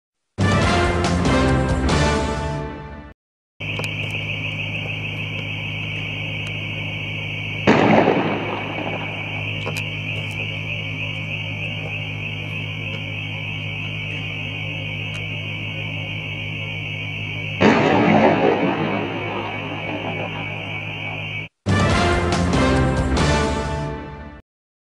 مرتزق يطلق الشوزن بشكل مباشر من المدرعة ويصيب أحد المتظاهرين 11/2/2014 Bahrain
أحد عناصر قوات المرتزقة الأعيرة النارية "الشوزن" من المدرعة بإتجاه متظاهر بشكل مباشر ويصبه بعدة شظايا استقرت في جسمه أثناء ااشتباكات وصدامات شهدتها جزيرة سترة يوم أمس الثلاثاء (11 فبراير/شباط 2014) بين مئات المتظاهرين وقوات النظام تزامنا مع اقتراب الذكرى الثالثة للثورة في البحرين.